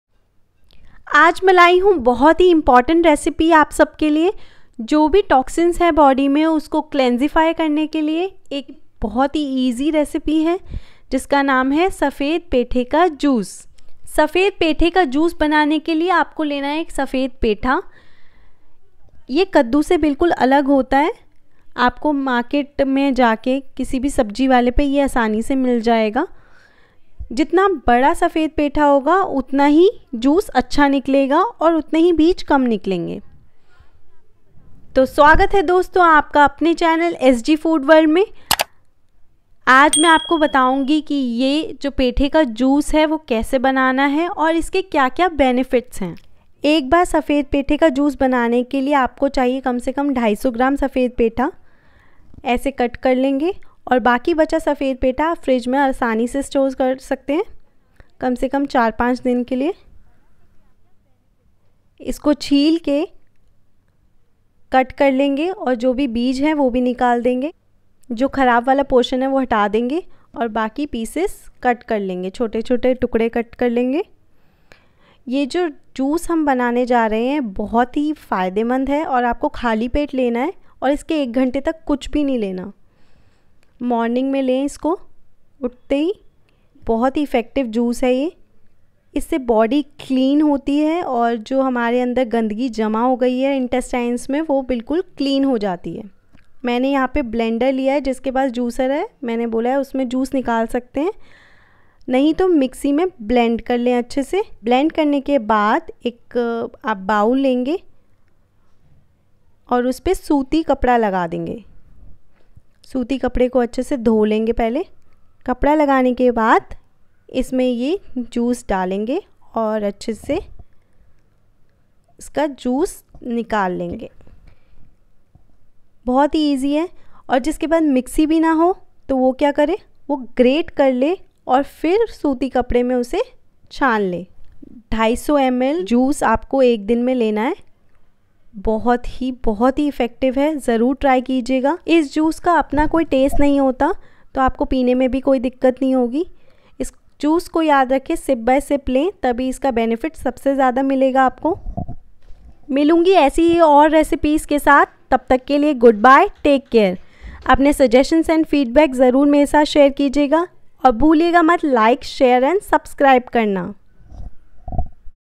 दुनिया का सबसे शक्तिशाली जूस कौन सा है? इस वीडियो में सीखे! | Most Powerful Detox Juice
इस वीडियो में आप सीखेंगे एक ऐसी सब्ज़ी के जूस के बारे में, जिसमें आपके शरीर के अंदर जमी गन्दगी निकालने की शक्ति बैठी है| एक ऐसा जूस जो चुम्बक की तरह, अंदर के सभी toxins को अपने अंदर खींचने लगता है| ऐसा जूस जो आपके शरीर के साथ-साथ मन्न को भी साफ़ करता है, बुद्धि बढ़ाता है| जानते है ये कोनसा जूस है? सफ़ेद पेठे का जूस!

1. सफ़ेद पेठे के जूस का फायदा क्या है?
2. इसको बनाने का सही तरीका
3. सफ़ेद पेठे के जूस के बारे में आपके सभी सवालों के उत्तर

15 दिन सुबह खाली पेट पेठा जूस पीने के फायदे जानकर हैरान रह जाएंगे आप Ash Gourd /पेठे का रस Benefits